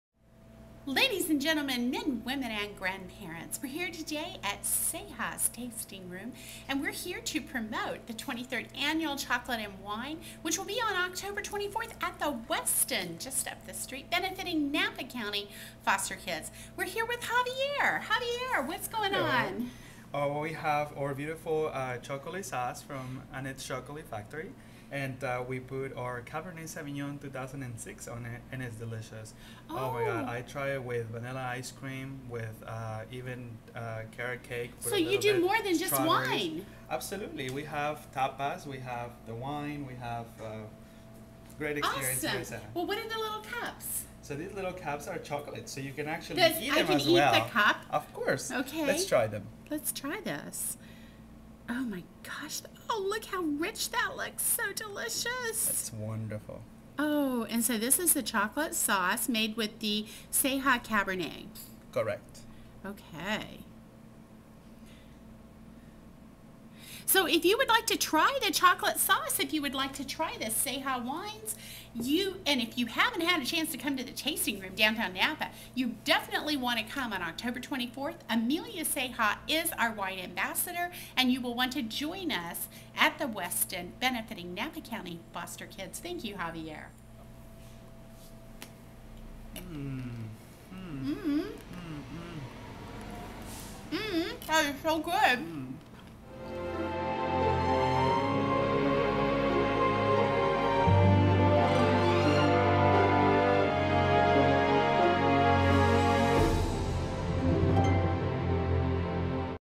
Chocolate Festival, Food and Wine, Napa Valley Ceja Vineyards, Doris Gentry
Enjoy 25 chocolatiers and 25 Wineries to benefit the Napa County Family and Foster Parent Association, Join Amelia Ceja, Wine Ambassador and Jan Taylor Chocolate Ambassador, Music by the Napa Valley Youth Symphony, Doris Gentry